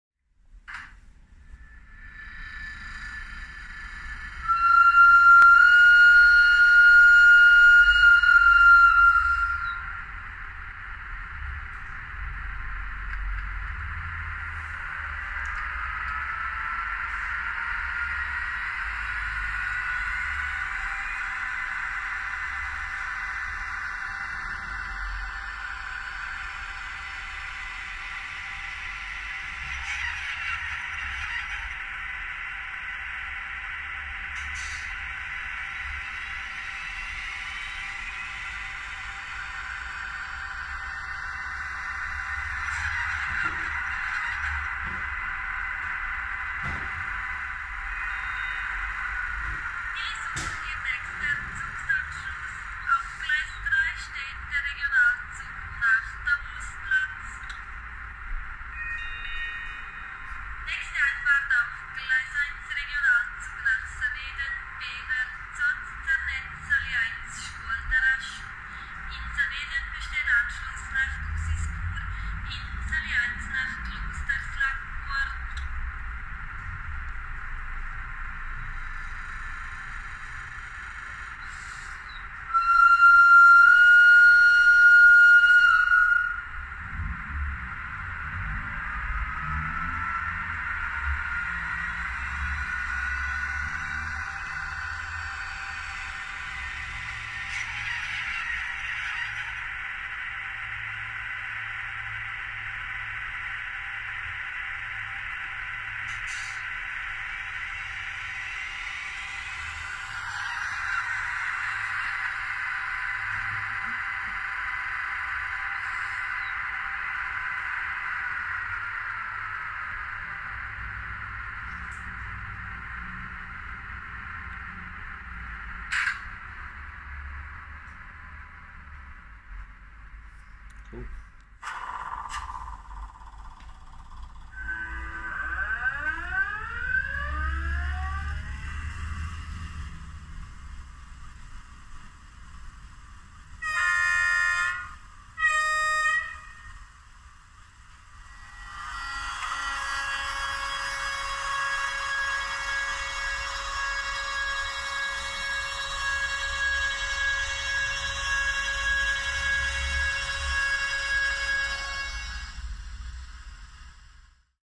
GE44
Hi everyone, the workshop is up and running full swing, and getting some of first sound fits since the start of lockdown completed.

One of the first jobs completed were these two superb looking H0m scale Swiss GE 4/4s!